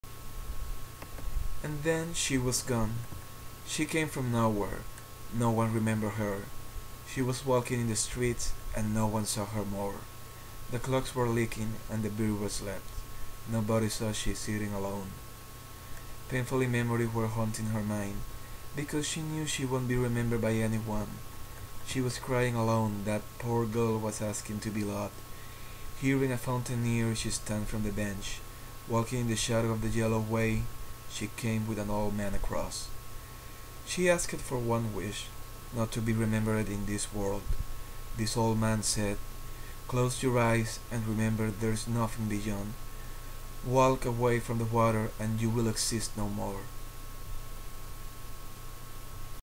And then she was gone...
And then...
she was gone...

By:

Gustavo R. Lequerica

She came from nowhere,
no one remember her,
she was walking in streets
and no one saw her more.

The clocks were liking,
and the birds were slept,
nobody saw she sitting alone,
painful memories were hunting her mind
because she knew, she won't be remember
by any one.

She was crying all alone,
that poor girl was asking to be loved,
hearing a fountain near
she stand from the bench,
walking in the shadow of the yellow way
she came with an old man across.

She asked for one wish
not to be remembered in this world,
the old man say " close you're eyes
and remember there is nothing beyond,
walk beyond the water and you will
exist no more".